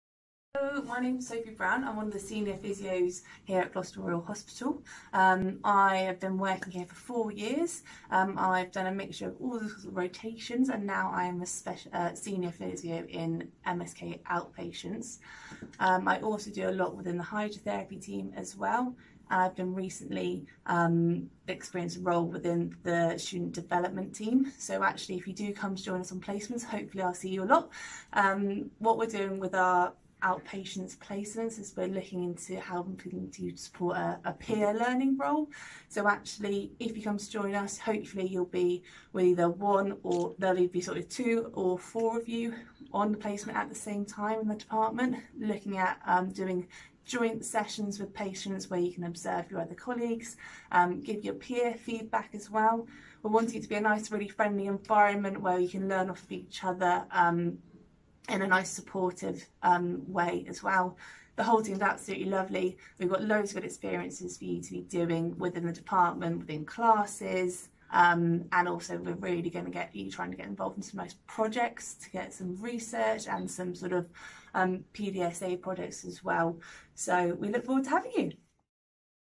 One Gloucestershire Clinical Placement Expansion Project
Sophie is one of the Senior Physiotherapists at Gloucestershire Royal Hospital, working mainly in MSK Outpatients, but also within the Hydrotherapy Team. She has recently taken on a role within the Student Development Team, supporting students on placement within the Outpatient Department in a peer learning role.